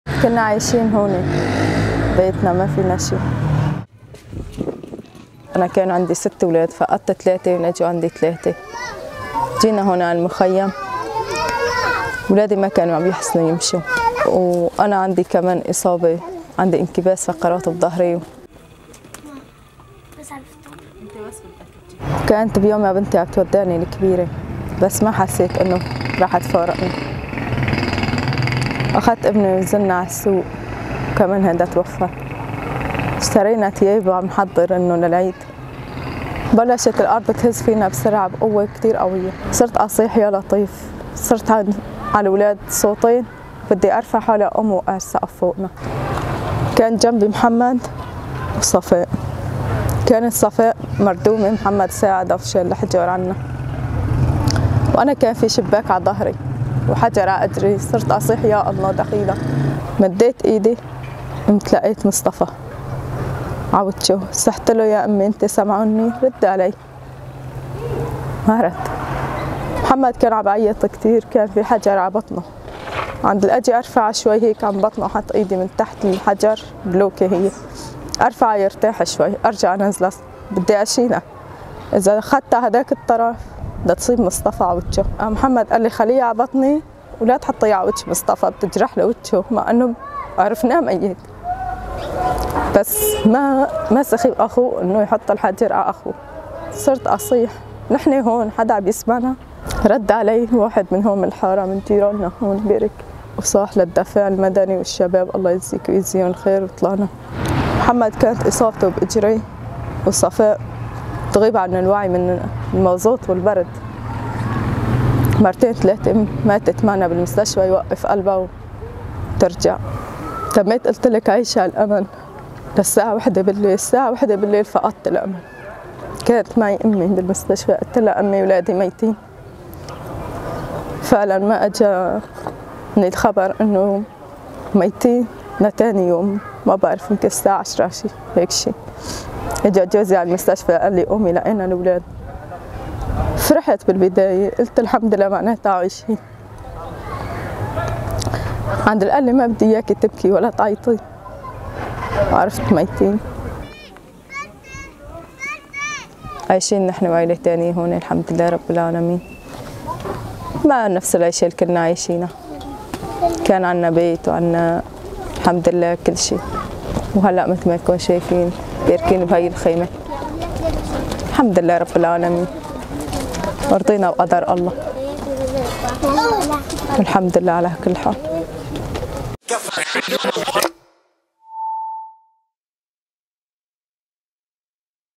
شهران على الزلزال :رواية إحدى الناجيات من ريف حلب
بعد شهرين على وقوع الزلزال في سوريا، تروي الناجية زينب نجيب من مدينة الأتارب في ريف حلب الغربي تفاصيل ليلة 6 شباط الفائت وما بعدها.